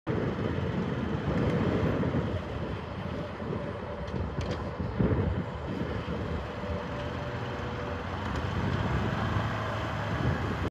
2019 POLARIS RANGER XP For Sale
OROPS, Crew Row, Manual Dump Bed, Winch on Back,  4x4, 2 Speed  Hrs. Showing 474, SN:  4XARVE875K8872354

 [2019 POLARIS RANGER XP 900 H.D.]